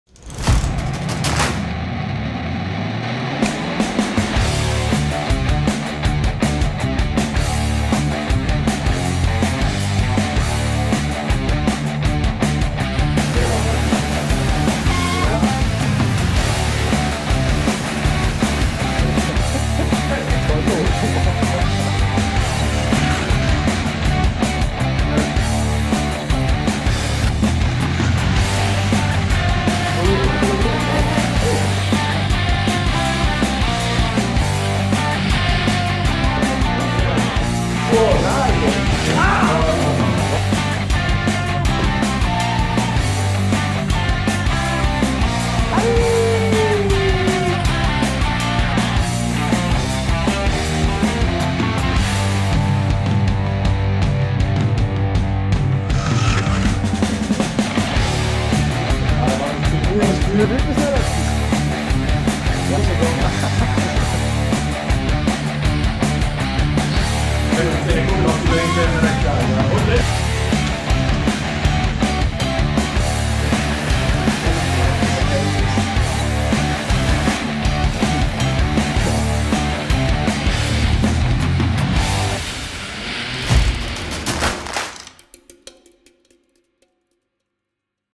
YB-Spieler duellieren sich auf der Carrera-Bahn
Mit dem Fahrrad Strom erzeugen. Die YB-Spieler versuchen sich darin und treiben die Carrera-Bahn mit Muskelkraft an.